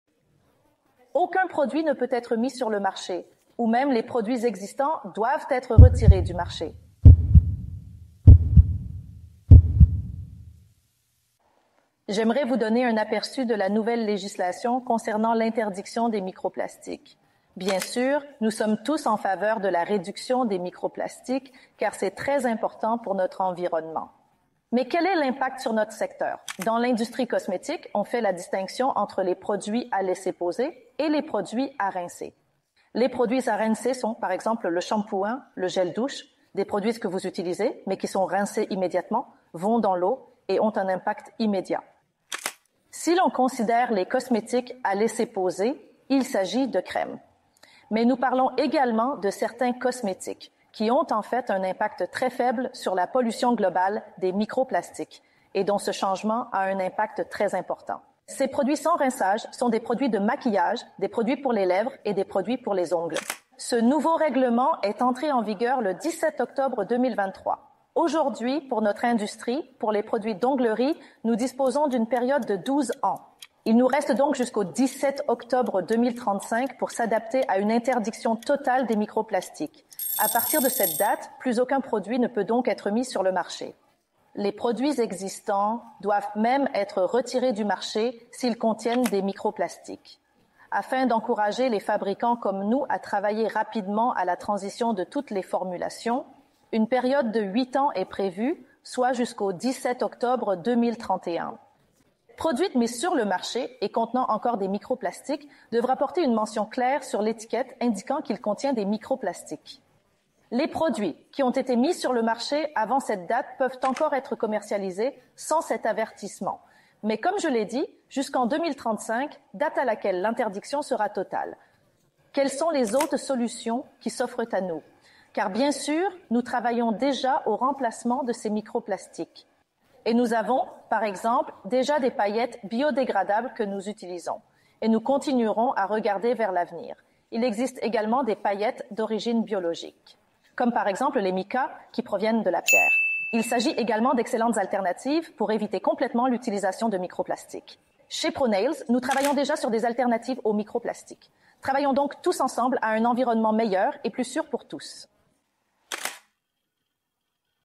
TECH TALK - Les paillettes vont-elles disparaître des salons de beauté ?
Quelle est la différence entre BFLEX et les autres bases colorées ? Birgit l'explique dans ce nouveau Tech Talk !